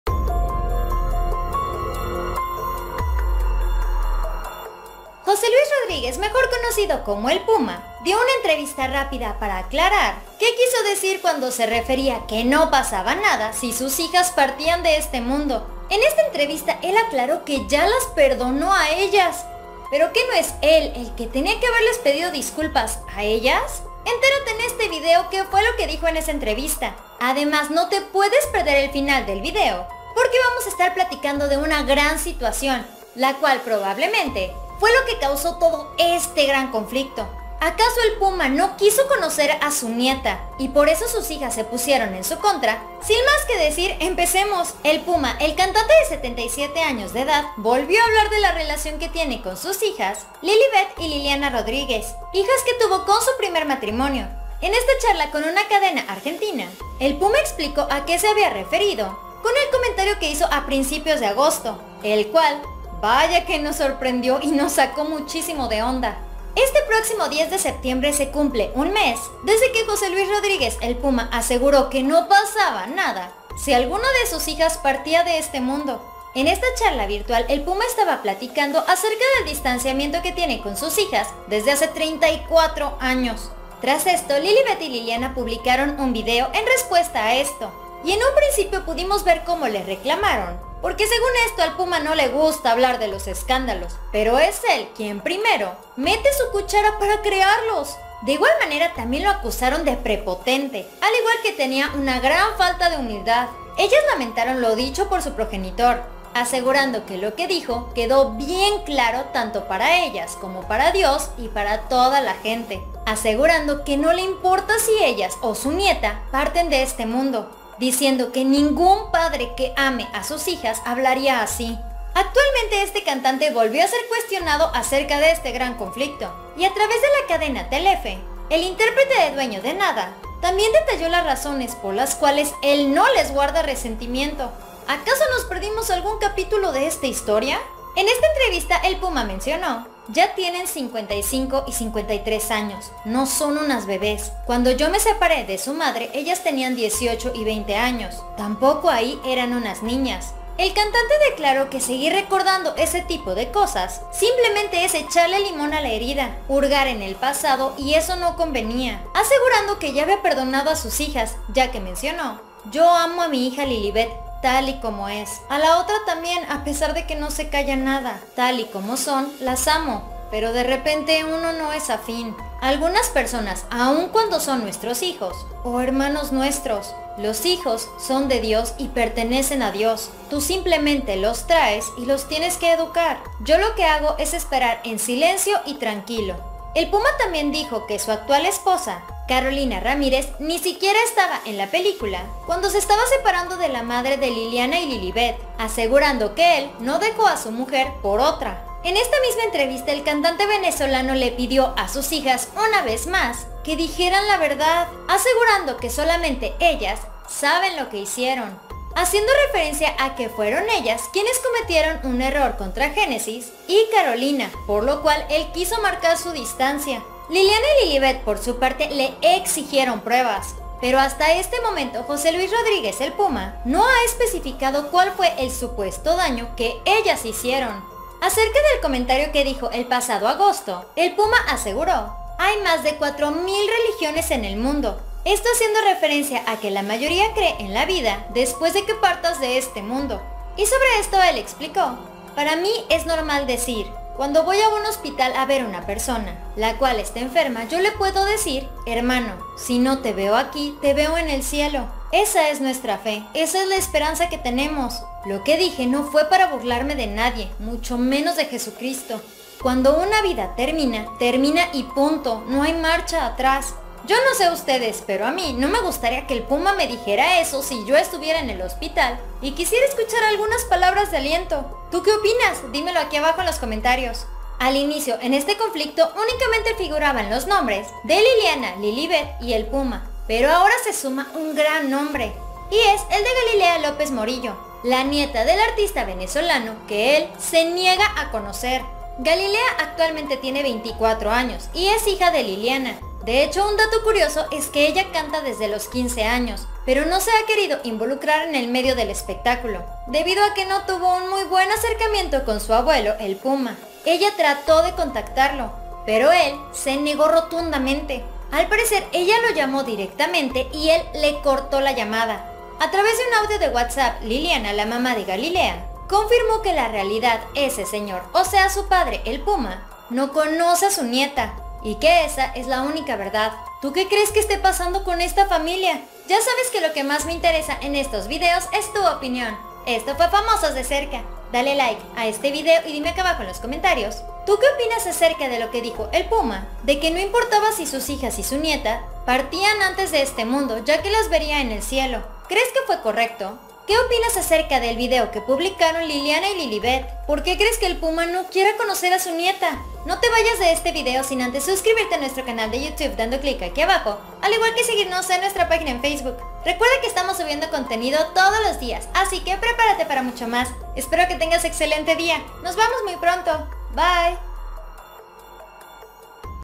🚨😱 ¡ El Puma Les 𝗔𝗰𝗹𝗮𝗿𝗮 𝗘𝘀𝘁𝗮𝘀 𝗖𝗼𝘀𝗮𝘀 a Sus Hijas ! 💥🔔
El cantante nuevamente les responde a sus hijas y les deja claro su amor.

 BIENVENIDOS!!!